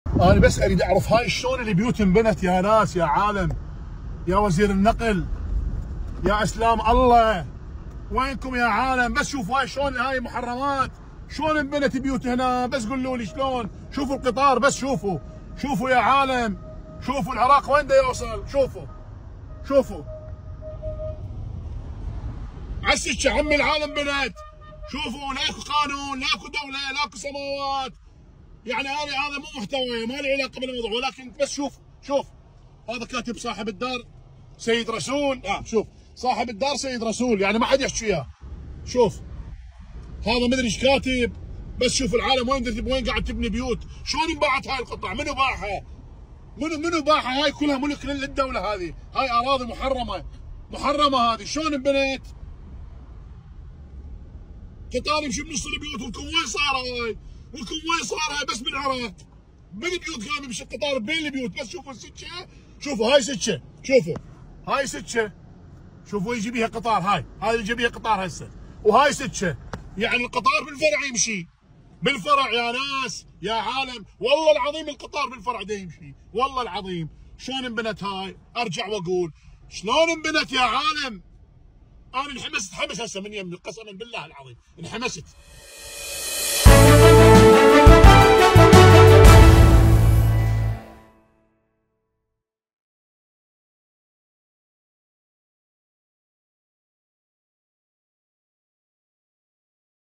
بناء بيوت بحرم السكة الحديدية يفجر غضب مواطن عراقي !
موقع قناة التغيير الفضائية  الالكتروني